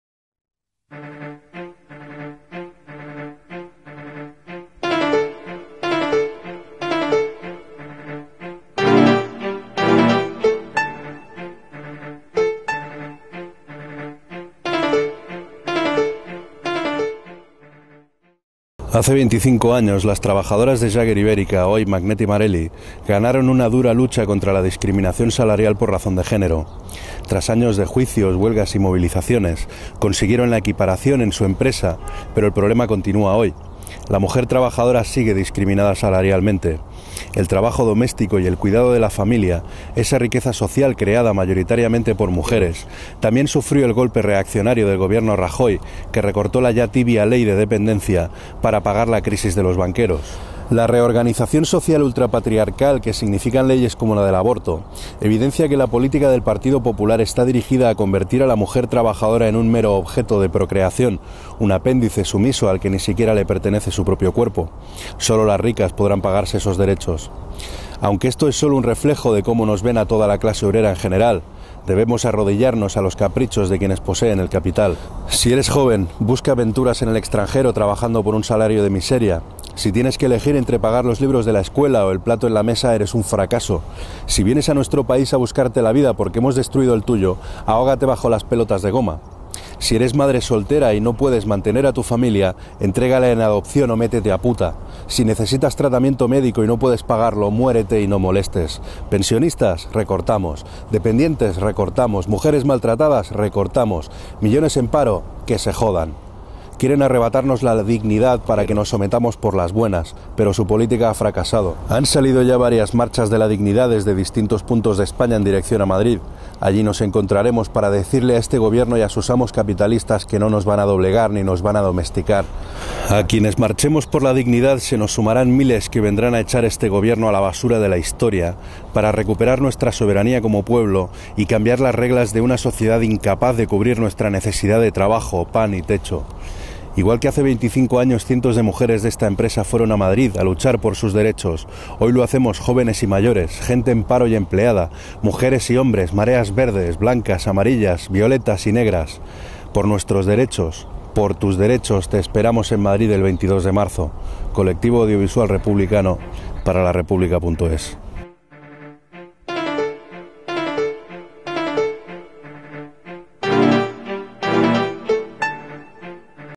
22 M Trabajo, Pan y Techo
Video noticia basado en un artículo de larepublica.es, sobre la marcha de la dignidad del 22-M hacia Madrid.
Video del colectivo audiovisual republicano.